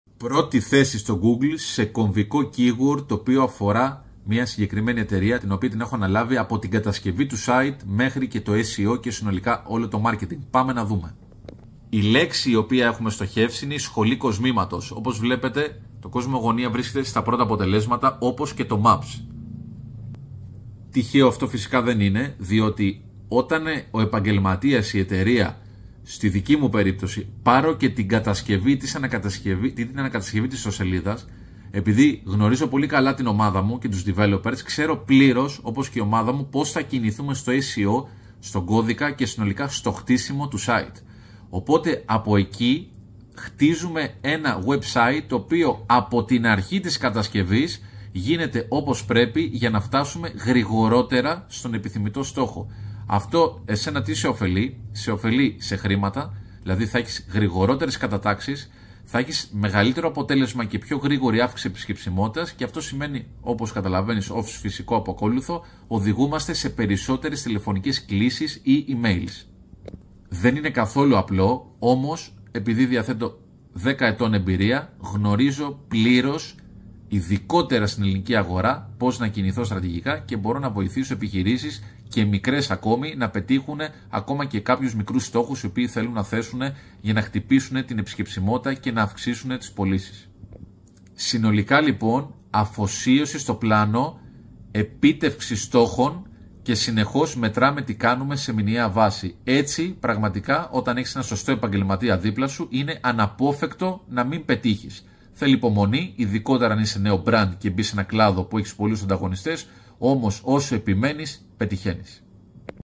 Πρώτη θέση στο Google σε κομβικό keyboard
💻 Η επίτευξη της πρώτης θέσης στα αποτελέσματα της αναζήτησης στο Google για κομβικές λέξεις-κλειδιά (keywords) είναι ένα απαιτητικό και συνεχόμενο εγχείρημα. Παρακάτω θα βρείτε μερικές βασικές συμβουλές για να βελτιώσετε την SEO (Search Engine Optimization) της ιστοσελίδας σας και να αυξήσετε τις πιθανότητες να φτάσετε στην πρώτη θέση στο Google για τα κομβικά keywords:

✅ Έρευνα λέξεων-κλειδιών: Αναλύστε ποιες λέξεις-κλειδιά σχετίζονται με το θέμα σας και έχουν υψηλή αναζήτηση από το κοινό. Χρησιμοποιήστε εργαλεία όπως το Google Keyword Planner για να βρείτε αυτές τις λέξεις-κλειδιά.
✅ Ποιότητα περιεχομένου: Δημιουργήστε υψηλής ποιότητας και πλούσιο περιεχόμενο που να είναι σχετικό με τα keywords που θέλετε να κατακτήσετε. Συμπεριλάβετε τα keywords στον τίτλο, τον κείμενο, και τα meta tags της ιστοσελίδας.
✅ Βελτιστοποίηση εικόνων και βίντεο: Αν μεταφέρετε περιεχόμενο πολυμέσων, βεβαιωθείτε ότι τα αρχεία είναι σωστά συμπιεσμένα και περιγράφονται με κατάλληλα keywords.
✅ Συνεχής ενημέρωση: Κρατήστε το περιεχόμενο σας φρέσκο και ενημερωμένο. Η τακτική δημοσίευση νέων άρθρων και πληροφοριών μπορεί να βοηθήσει στην αναβάθμιση της θέσης σας στα αποτελέσματα της αναζήτησης.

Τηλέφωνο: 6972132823